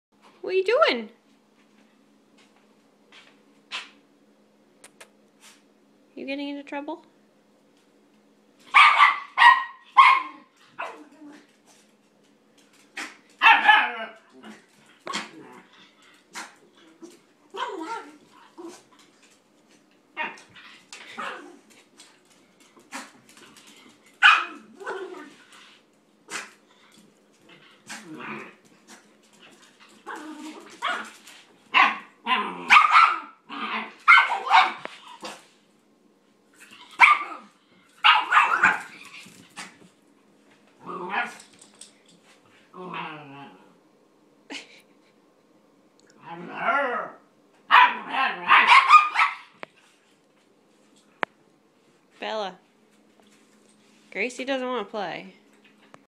9 week old German Shepherd Puppy and my old Toy Poodle
Our new puppy Bella trying to play with Gracie, our 11 year old toy poodle, who was not impressed..